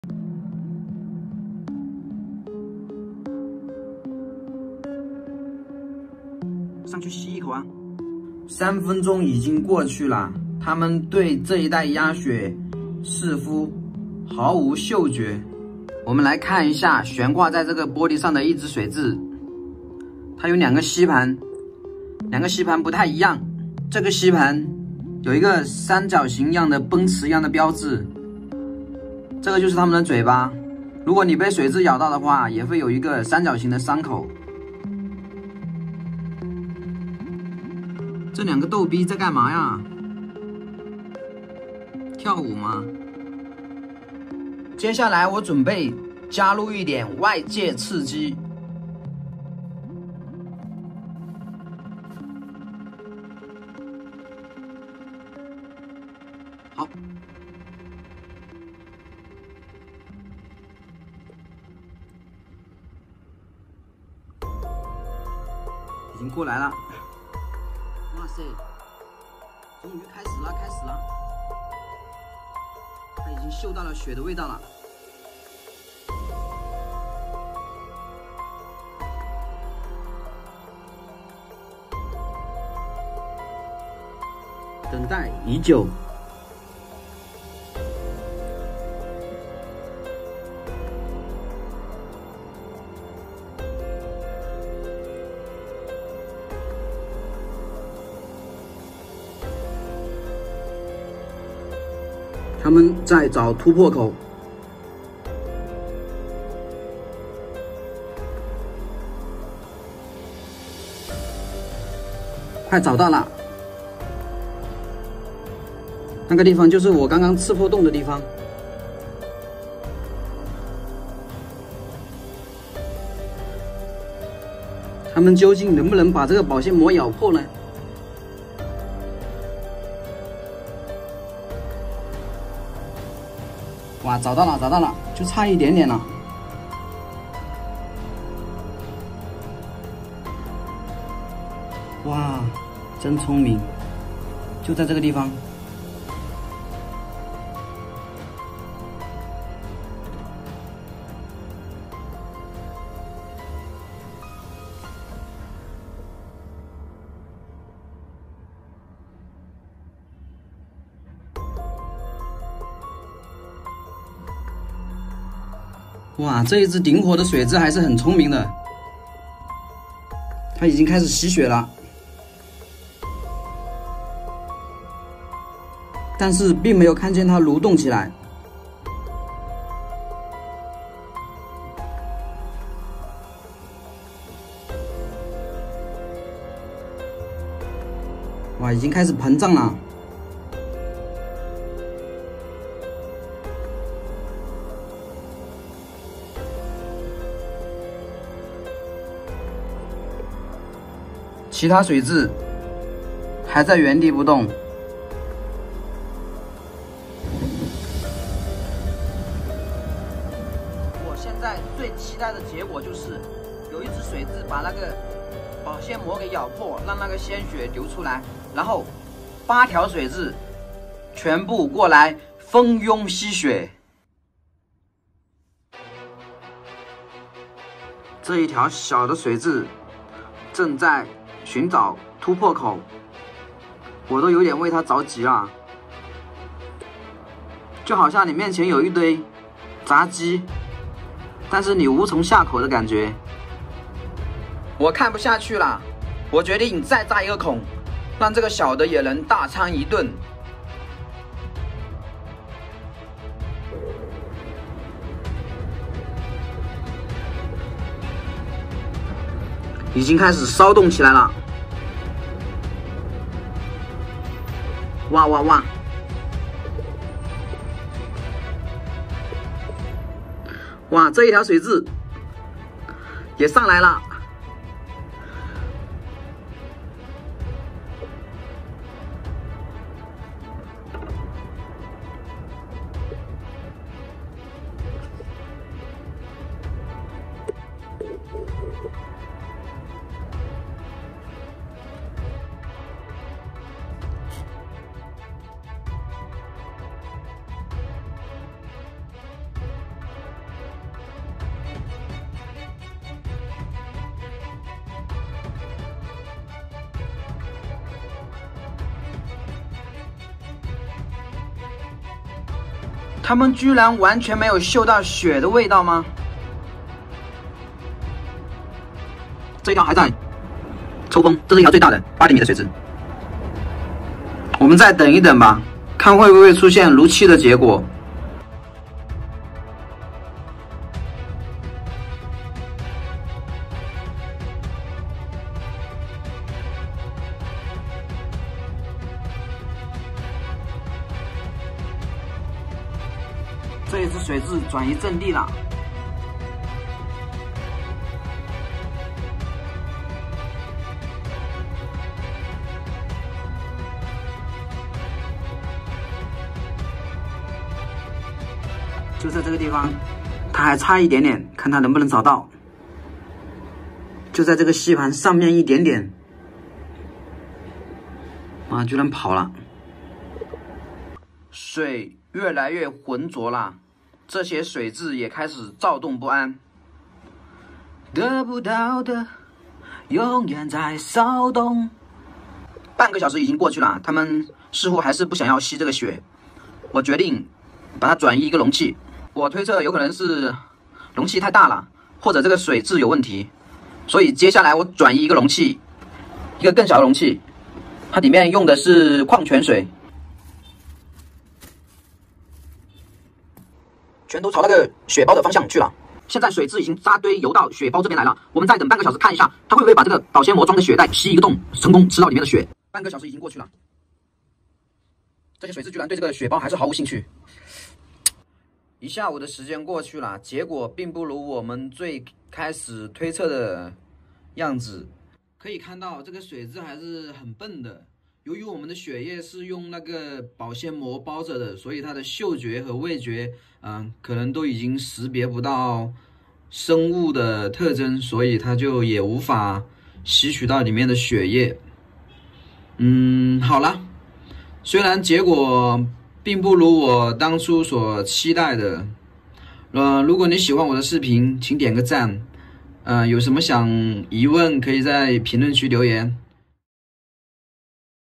WHAT HAPPEN YOU PUT INTO BAG BLOOD TO 8 HUNGRY LEECHES?
hello everyone, This video is an assignment given to me by my biology teacher, recording and observing the blood-sucking behavior of leeches - How Leeches Bloodsucking Works